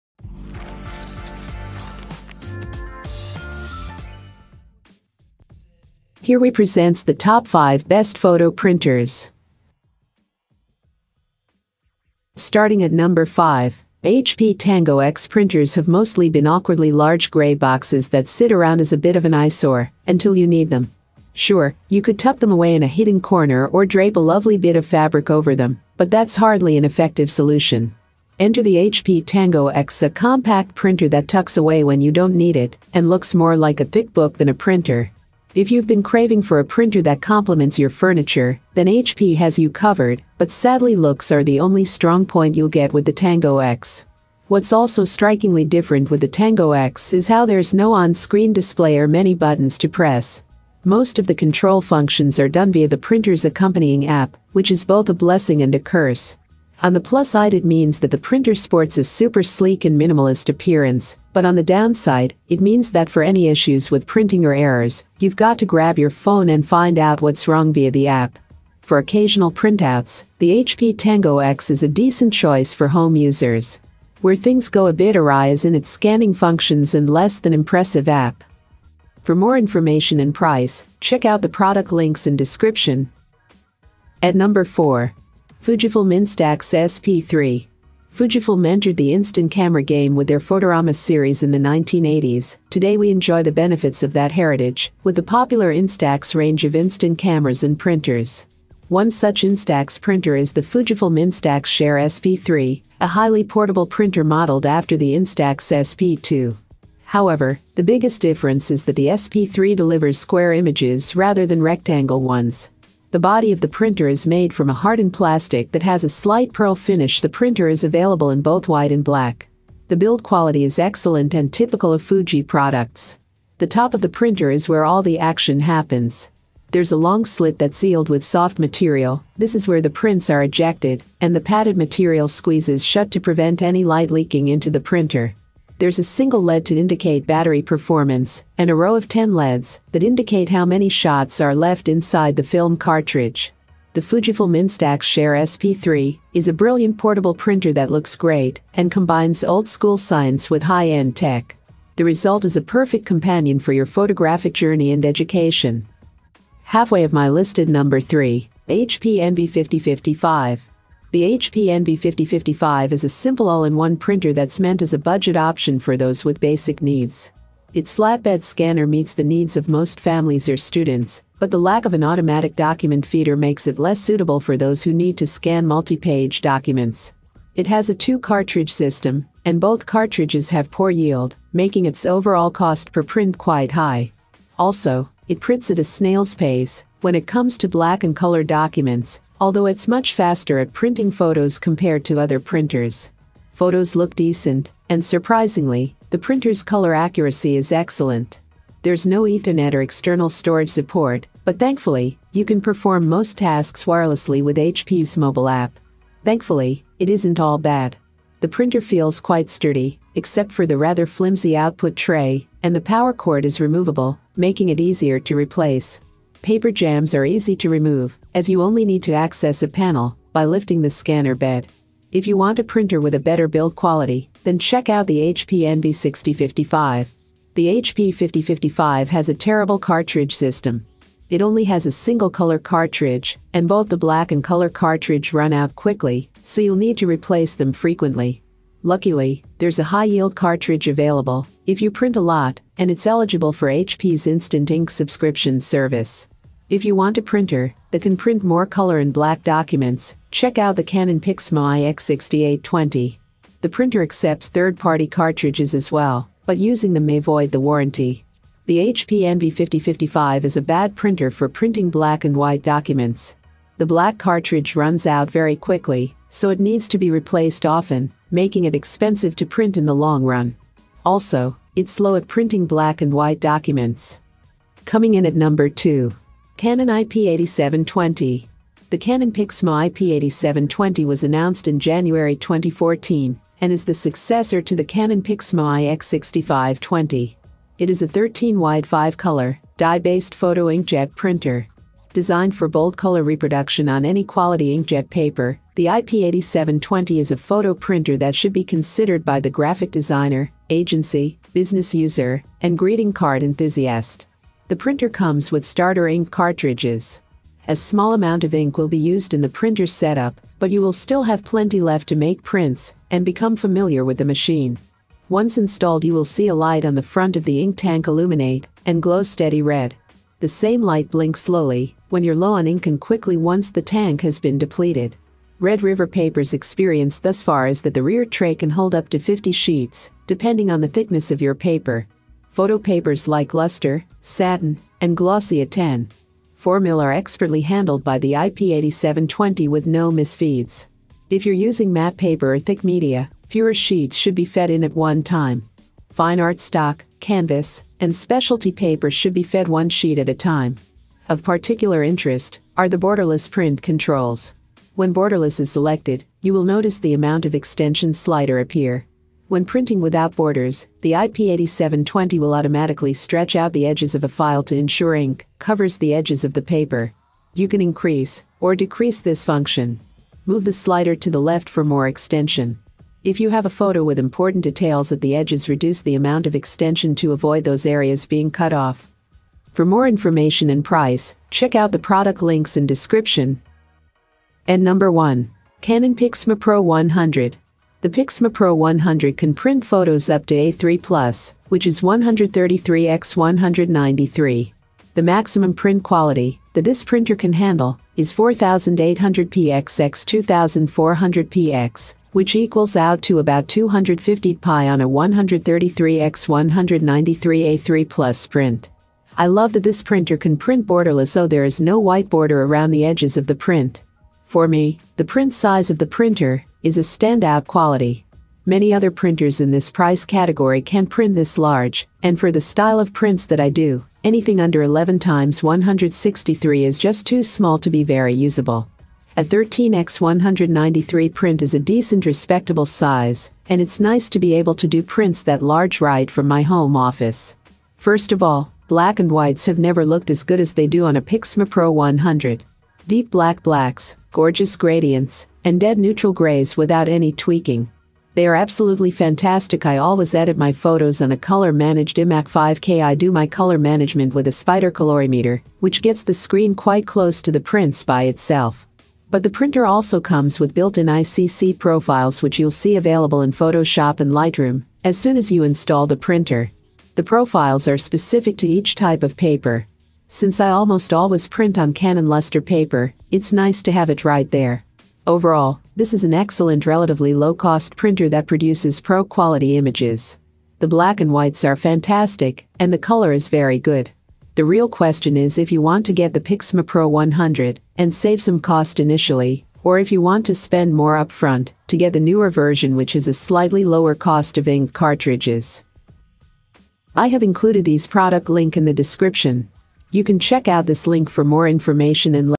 ✅ Best Photo Printers 2023 - Top 5 BestPhoto Printers
Here is the Updated Prices of the Best Photo Printers in 2023!

Things that we mentioned in this video:
1. Canon PIXMA Pro 100 - B0095F5BCS
2. Canon IP8720 - B00HM0IV52
3. HP ENVY 5055 - B0799D4T89
4. Fujifilm Instax SP 3 - B076DRQ5XB
5. HP Tango X - B07HRG69PR